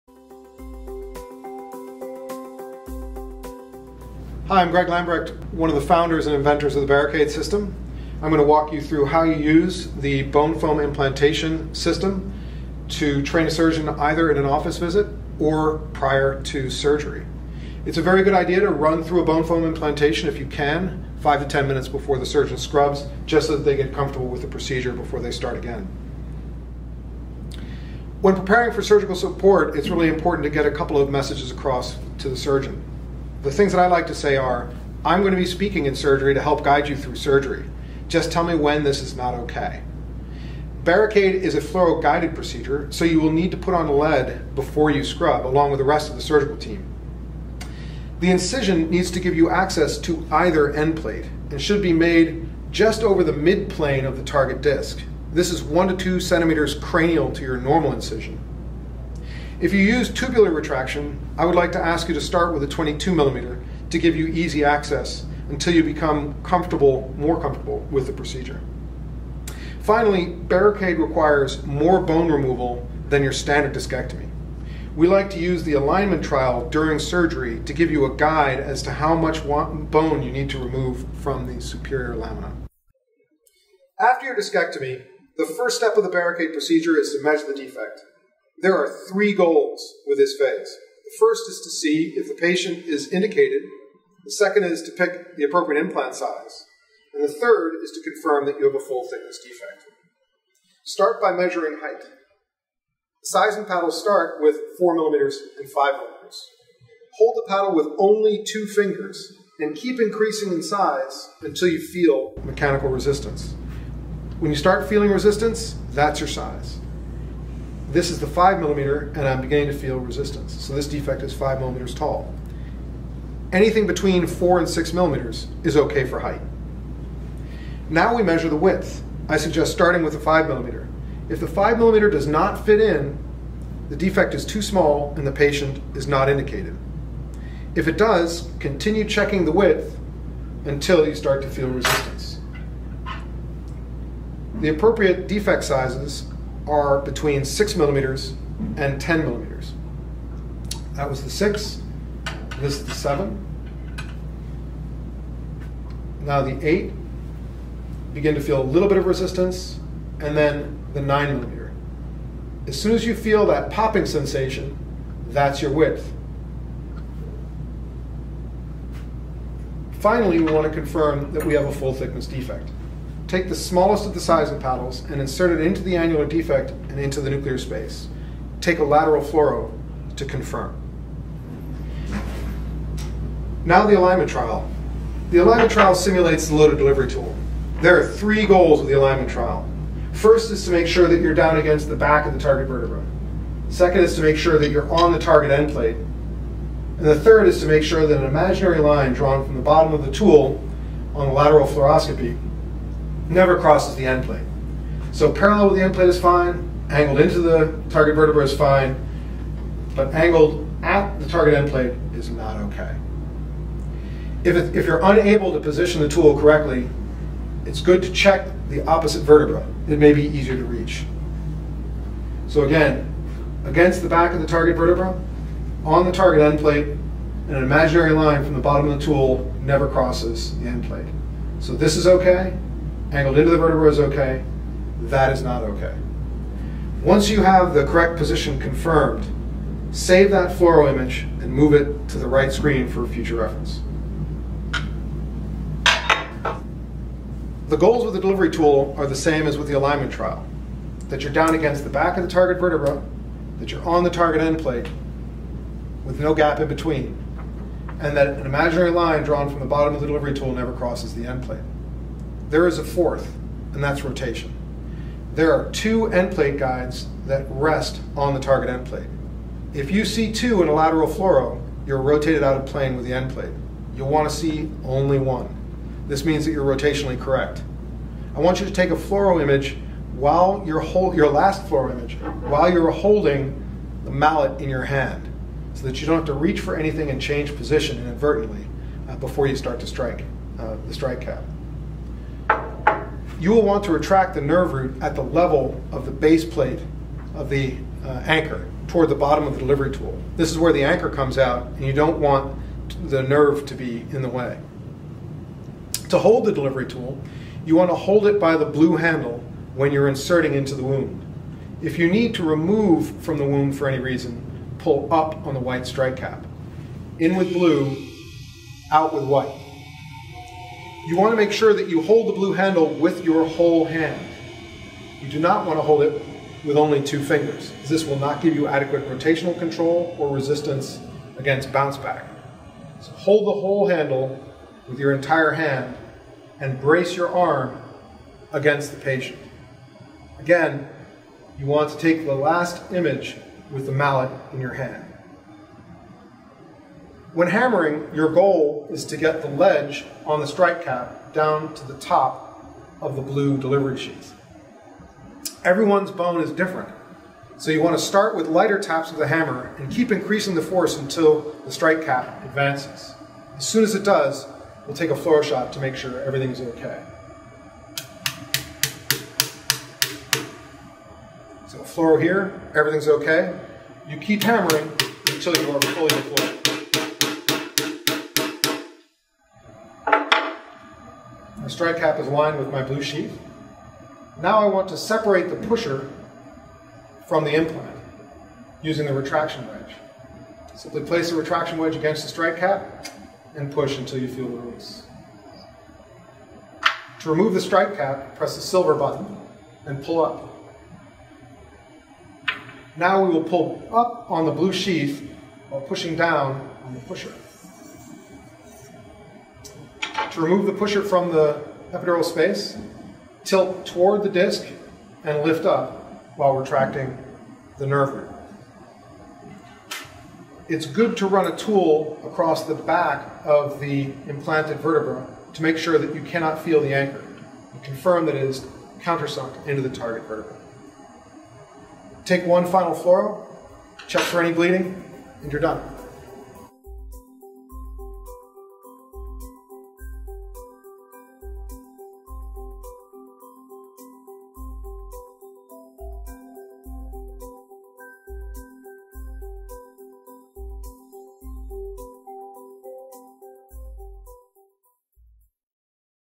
Sawbone Demo - How it Works
The Barricaid demo kit includes a sawbone model intended to demonstrate proper technique for implantation of the Barricaid.

In this video, founder Greg Lambrecht shows you how to conduct a technique demonstration with sawbones.
VID004 Rev. A